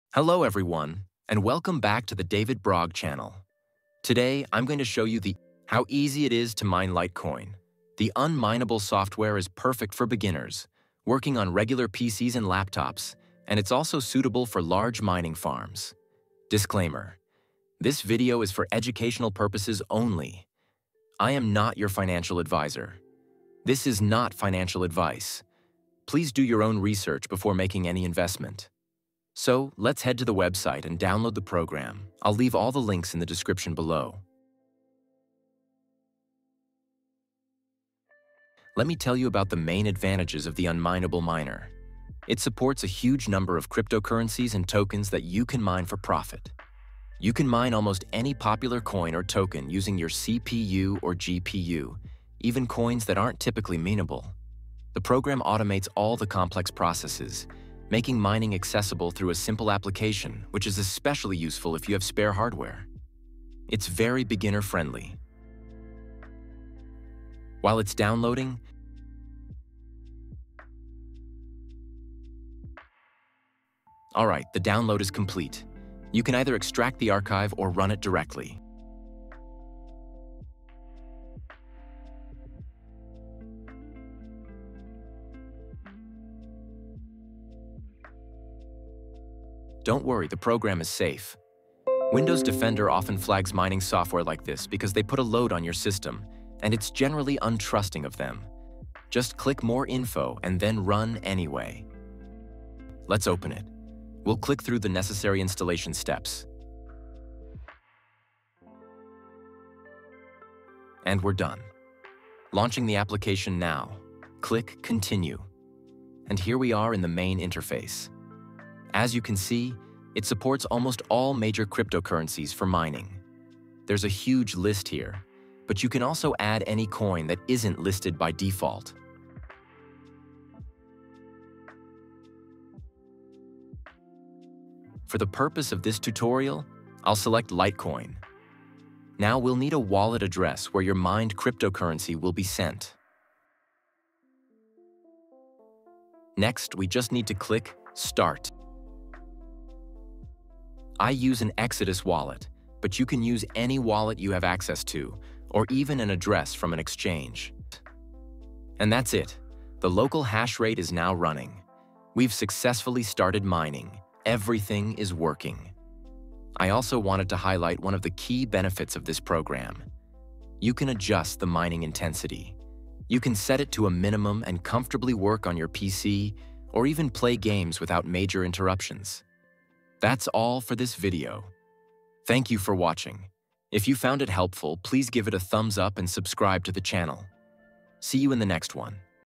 How to Mine Litecoin (LTC) in 2025 WITHOUT an ASIC! (unMineable Guide)
Think you need a expensive, loud ASIC miner to get Litecoin? Think again! In this video, I'll show you the secret to earning Litecoin (LTC) on your own computer using the unMineable platform. This is the easiest and most accessible way to start building your LTC holdings in 2025.

unMineable works by letting you mine other, more GPU-friendly cryptocurrencies and get paid automatically in Litecoin. I'll walk you through the entire process from start to finish in this complete, step-by-step beginner's guide.

📖 IN THIS VIDEO:
00:00- Intro: The Secret to Mining Litecoin on a PC (What do we need?)
00:45- Installation
02:00- I'm showing what a huge number of cryptocurrencies it supports
02:15- Add the address Litecoin from your wallet to which the mined cryptocurrency will be sent.
02:40- We launch it and see that the process has started, the hashrate is displayed, which means everything is working.